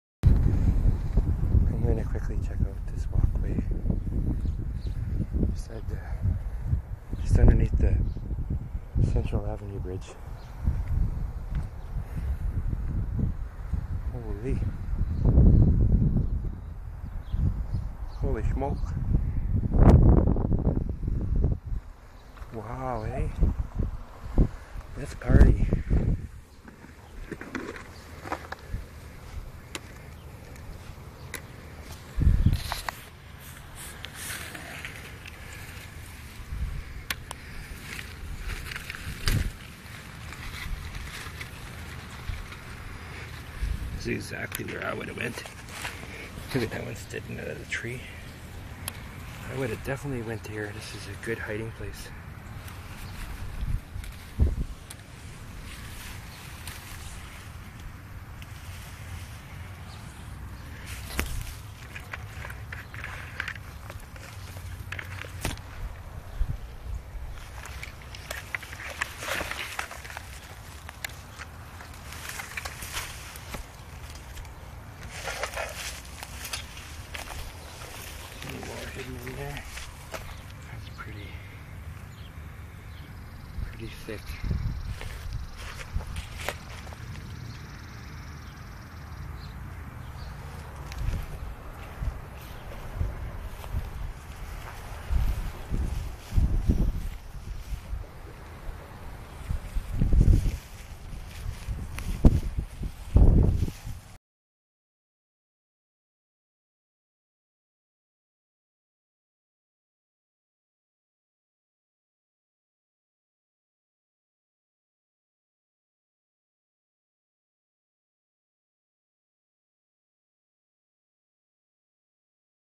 Needle Picker Central Avenue cleanup part 2 of 2
It seems like every shelter from eyeballs is littered by needles in my home city of Prince Albert Saskatchewan.